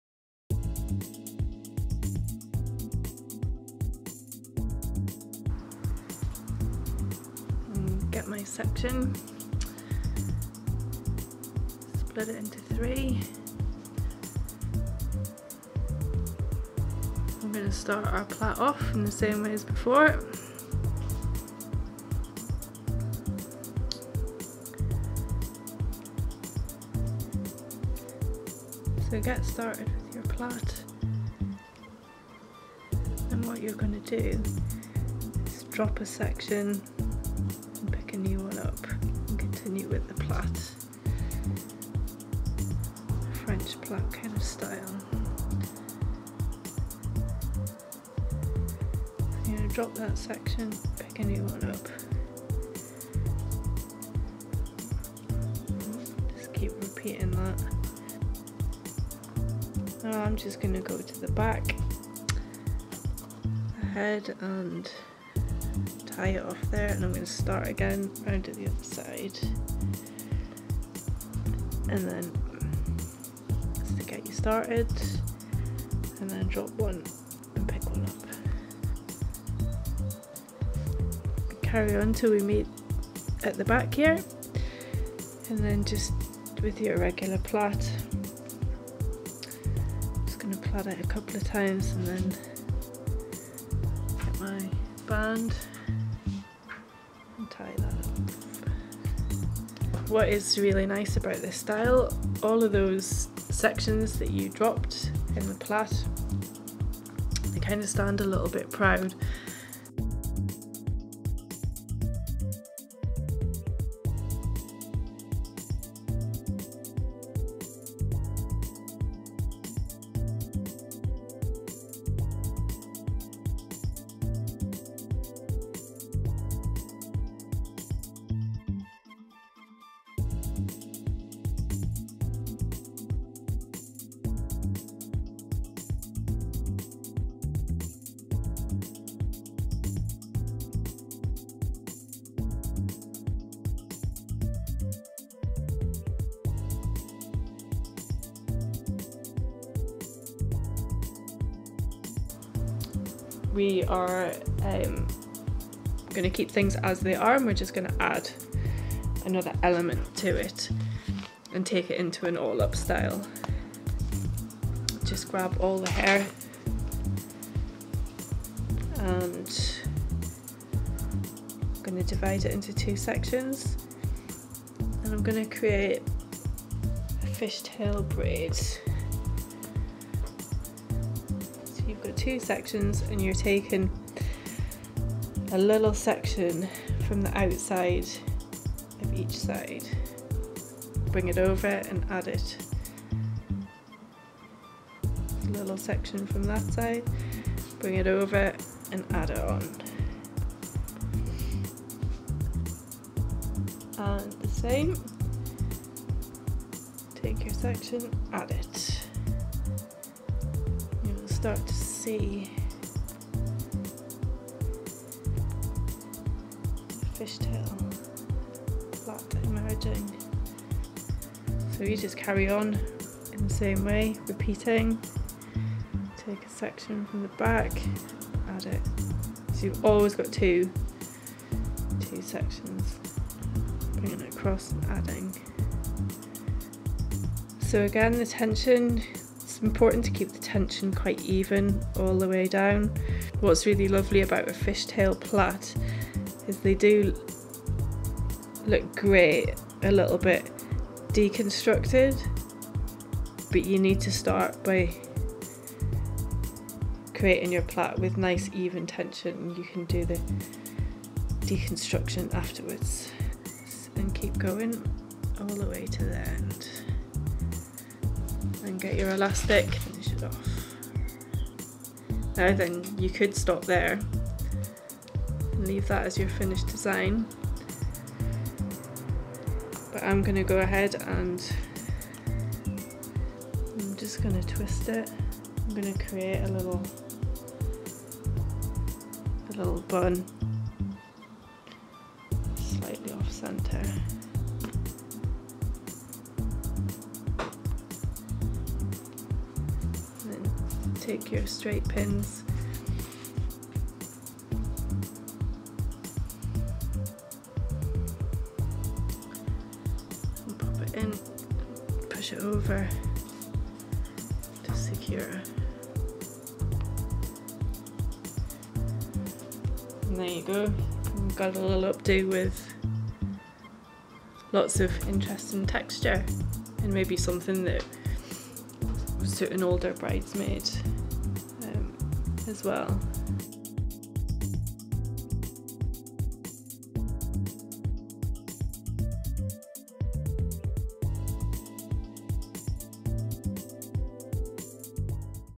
How to combine a waterfall and fishtail braid together
Workshops: Group bookings: 1-1 Tutorials: Occasions: Weddings: Photo shoots: Editorial: Television: Advertising

Model Katharine McLure